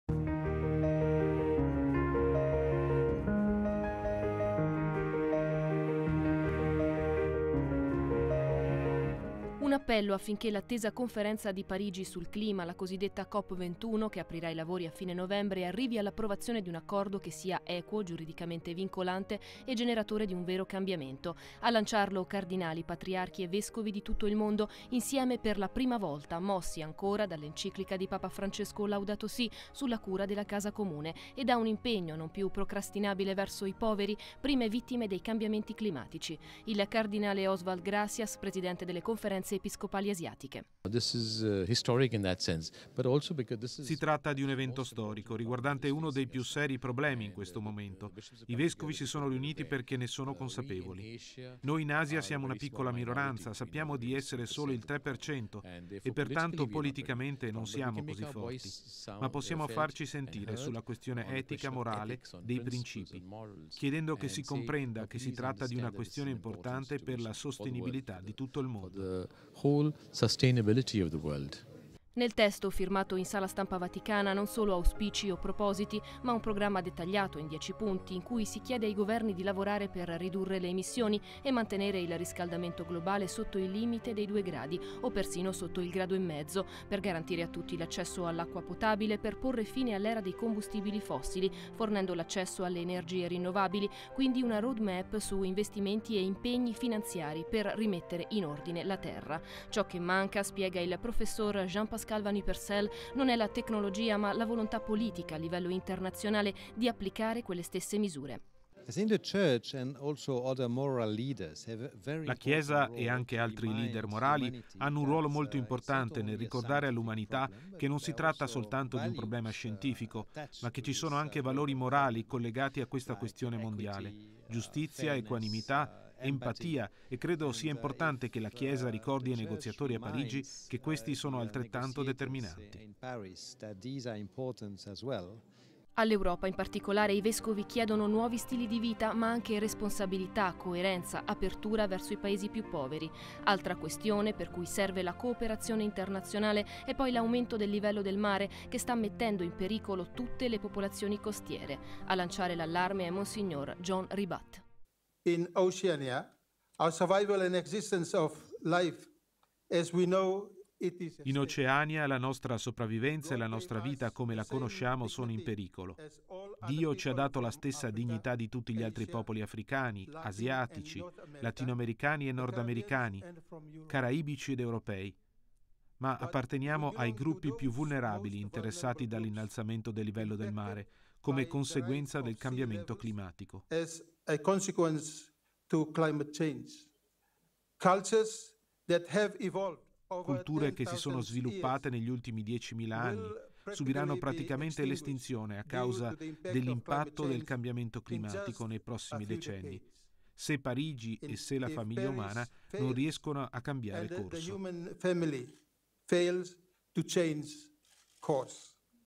Vatican Magazine " La Chiesa unita per il Pianeta " 06-11-2015
L’appello di cardinali, patriarchi e vescovi di tutto il mondo perché la conferenza di Parigi sul clima, a fine novembre arrivi ad un accordo “generatore di vero cambiamento”. Il cardinal Gracias. “E’ un momento storico per la Terra”.